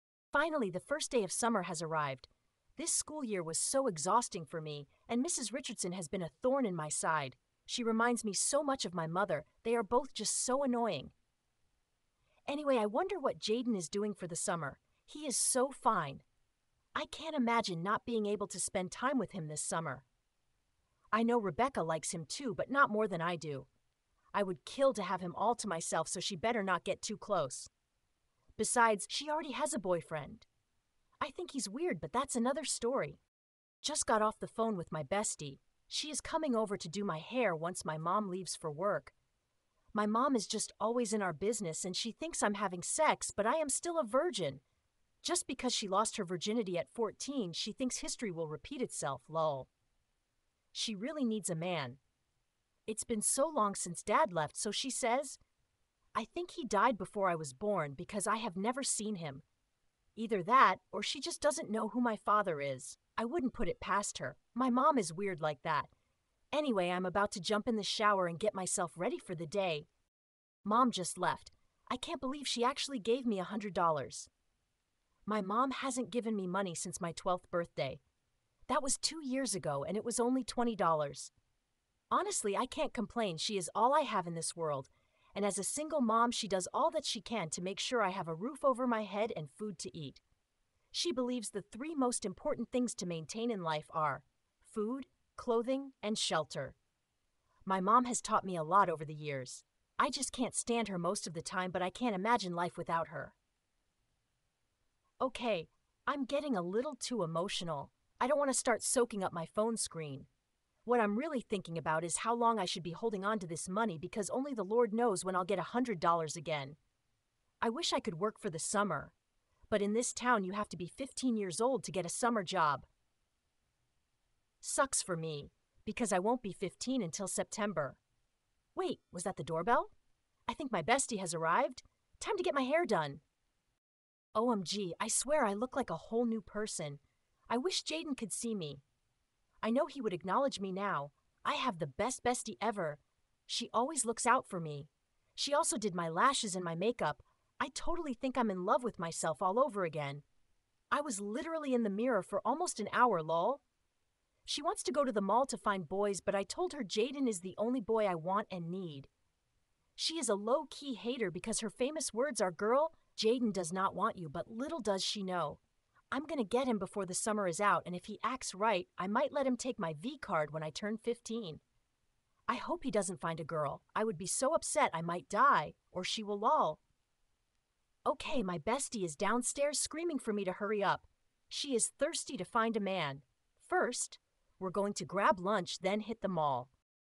Chapter 1: Summer Has Arrived
Dive into the mind of a teenage girl through her diary as she navigates the most thrilling and perilous summer of her life.
MIDSUMMER DIARIES: The Confession by JOSEPH L LEATH